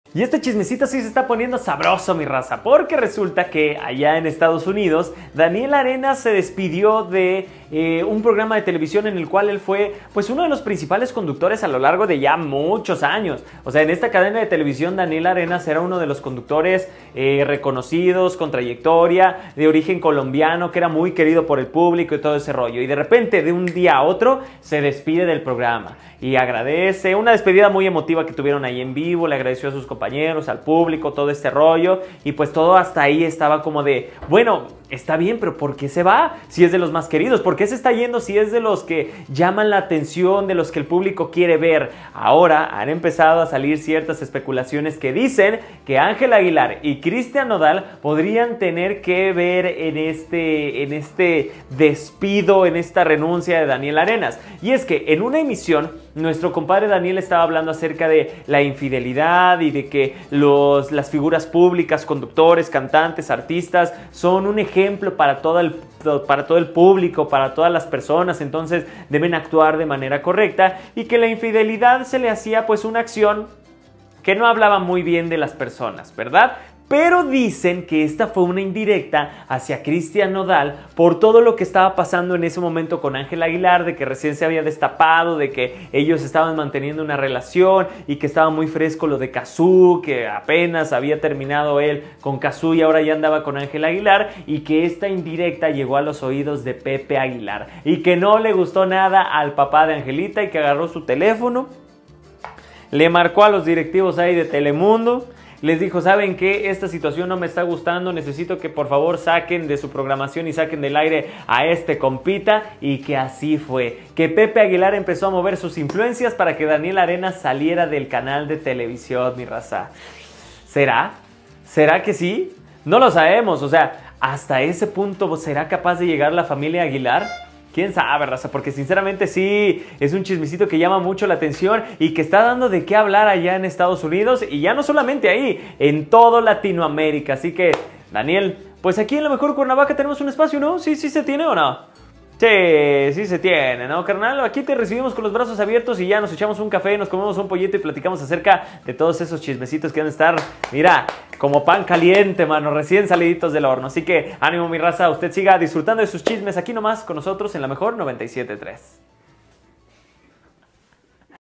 ¡Por decir la verdad! ¿Despiden a Daniel Arenas por culpa de Ángela Aguilar y Christian Nodal?
DanielArenas “ Referente 😒 con AngelaAguilar , ChristianNodal 🙄 y Cazzu por eso está la sociedad como está 😑 porque estamos normalizando que quien ama 💔 puede ser infiel”